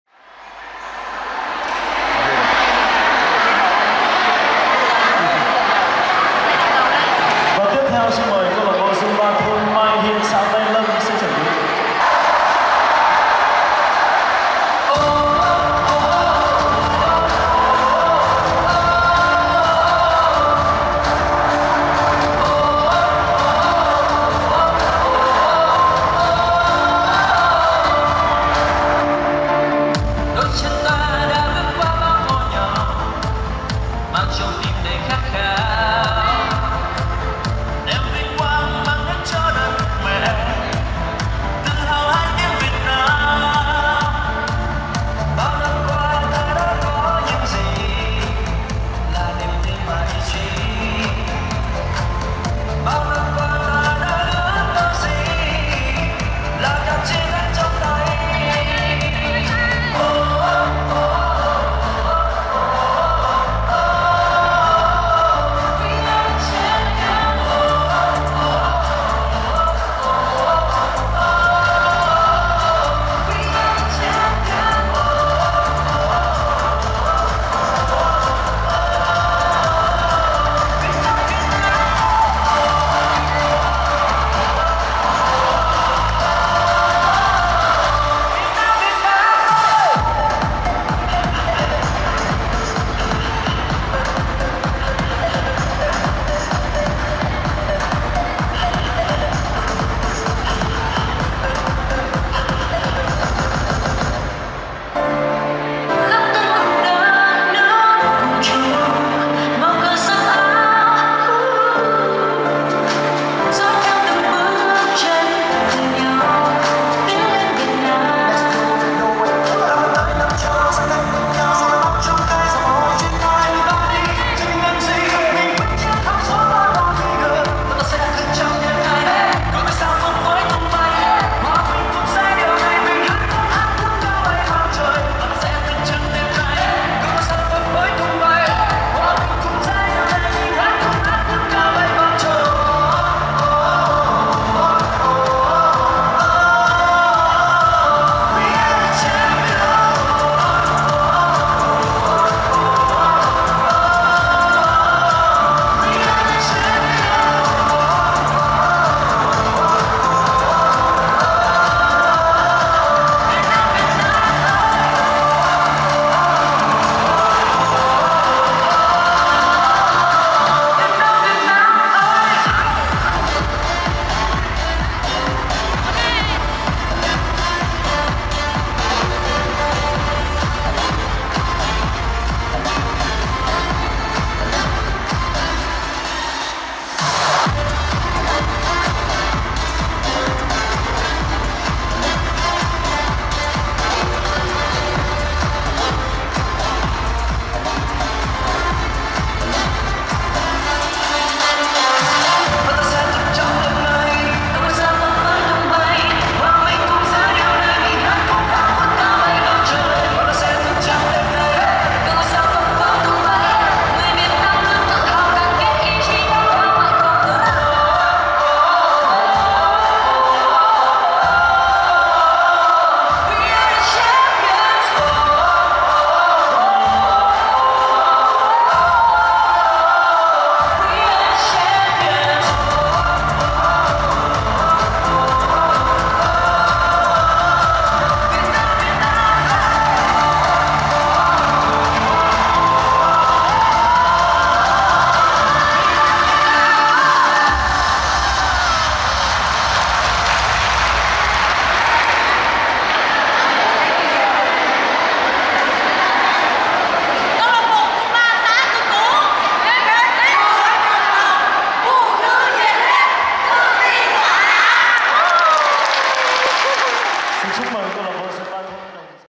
Zumba XÃ DỤC TÚ - CHUNG KẾT CUỘC THI Zumba GẮN KẾT CỘNG ĐỒNG H. ĐÔNG ANH - GIẢI 3
Chung kết cuộc thi Zumba gắn kết cộng đồng do LHHPN huyện Đông Anh tổ chức. Cuộc thi diễn ra tại nhà thi đấu huyện Đông Anh thật sôi động và thực sự mang lại sức khỏe,niềm vui, sự tự tin trong cuộc sống của các thành viên.